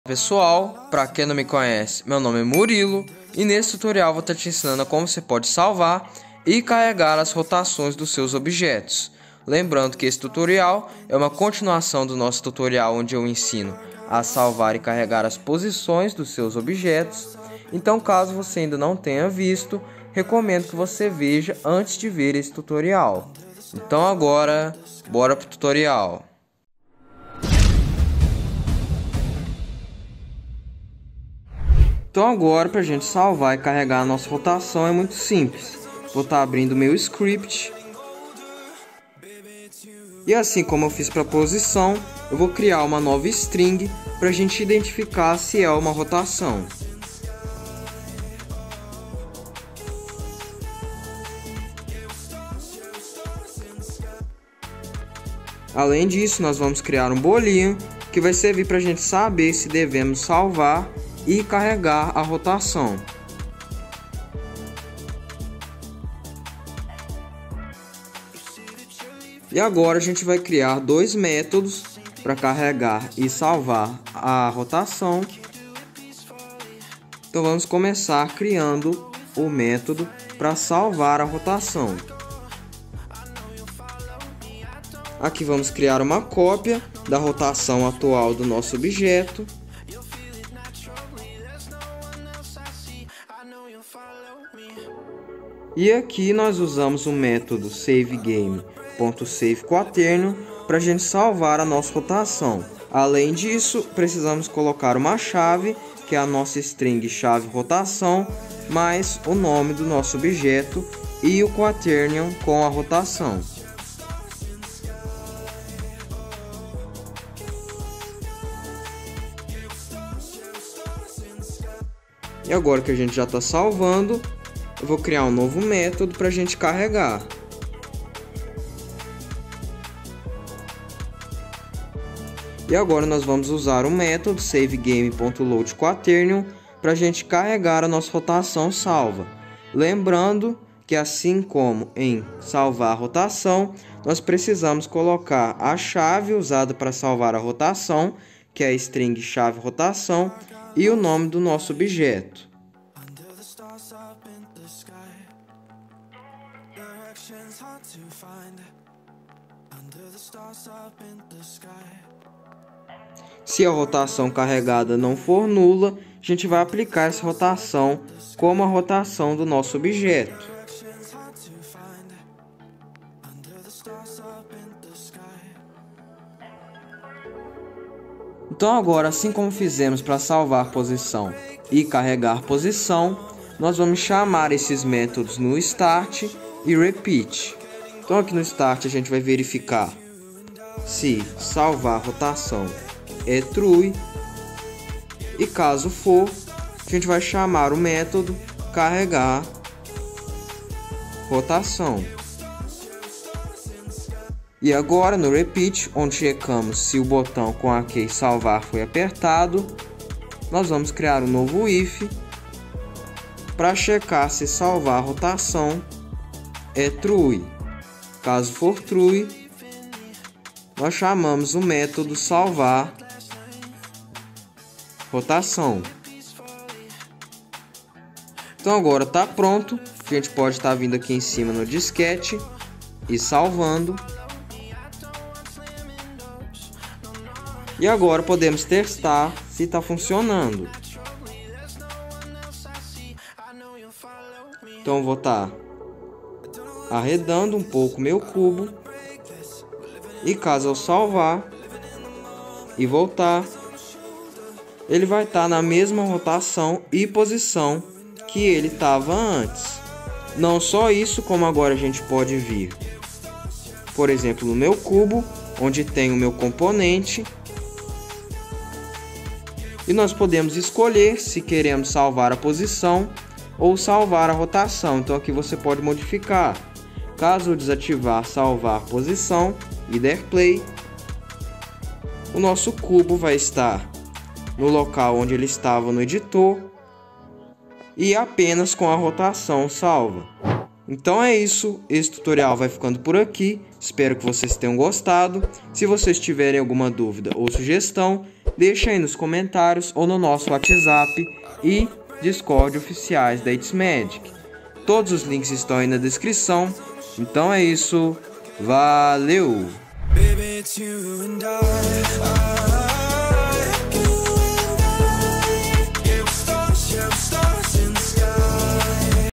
🔄 Como Salvar e Carregar Rotações de Objetos na ITsMagic [PARTE 2]
Neste tutorial, você aprenderá a salvar e carregar as rotações dos objetos usando a classe SaveGame na ItsMagic Engine. Veja o passo a passo detalhado para gerenciar rotações de forma eficaz no seu jogo.

Assista esse video por dentro da aba tutoriais da itsmagic para ter uma experiencia melhor e poder aplicar o conhecimento junto comigo =]

Nivel: Básico